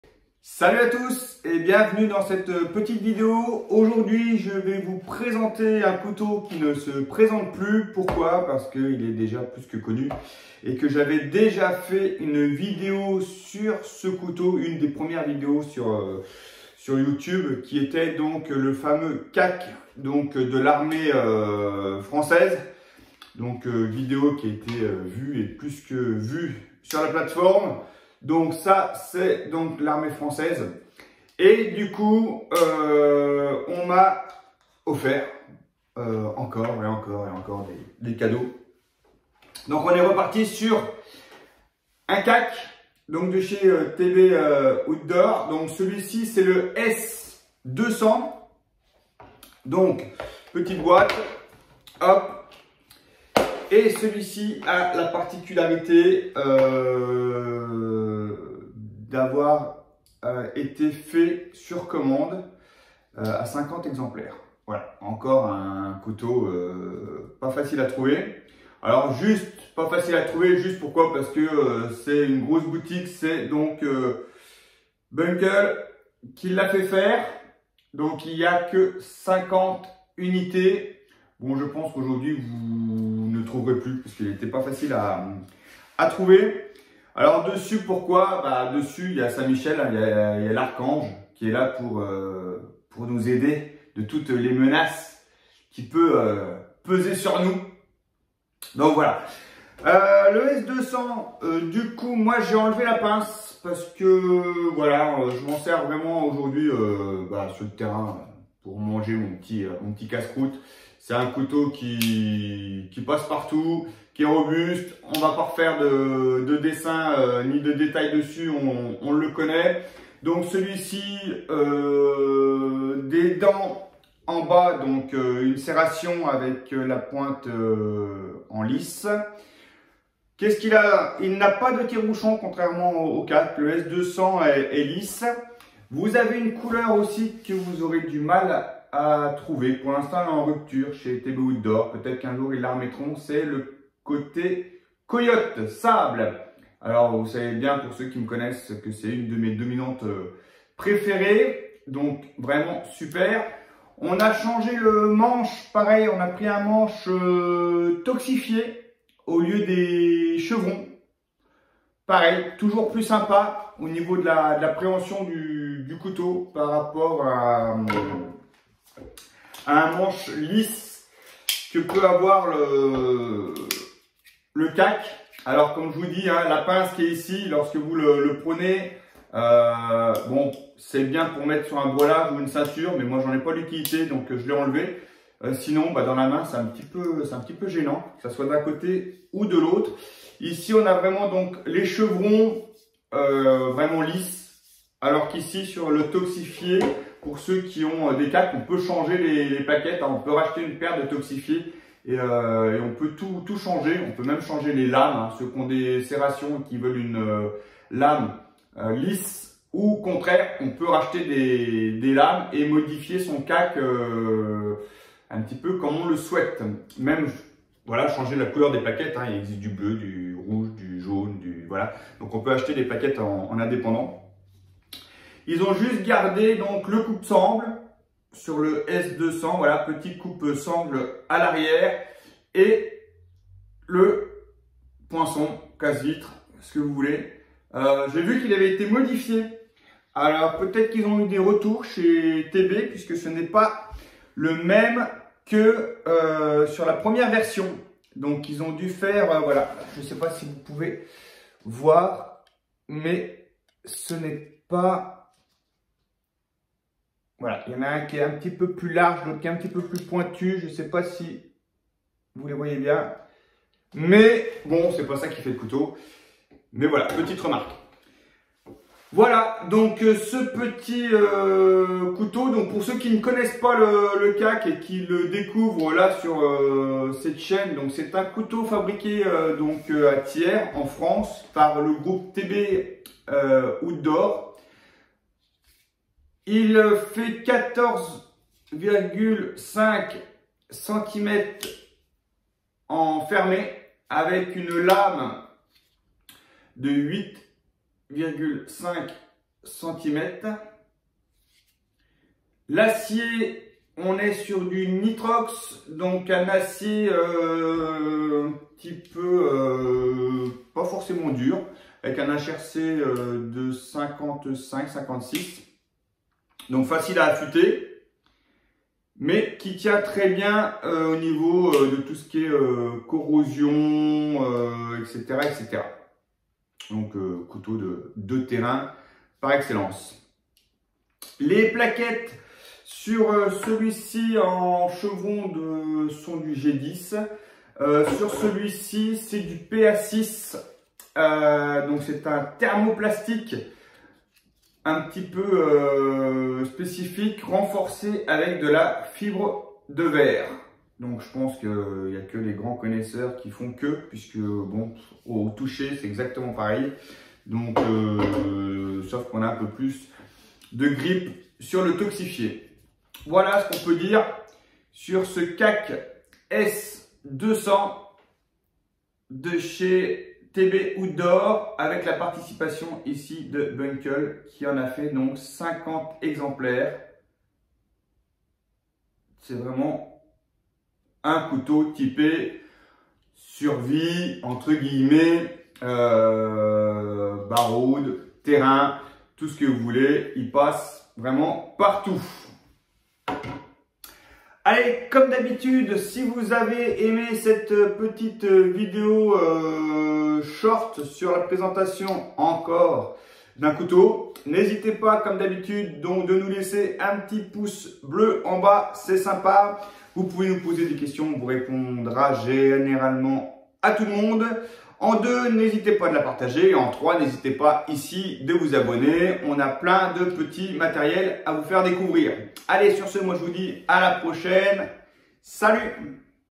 CAC S200 TB-Outdoor, le MUST du pliable sur le terrain. 50 exemplaires !!
On ne le présente plus, mais l'indéniable C.A.C S200 est de retour avec encore une particularité d'une série limité à 50 unités, réalisé par la boutique de survie BUNKL. la sérigraphie représente l'archange st Michel nous défendant de toutes les menaces qui nous entoure.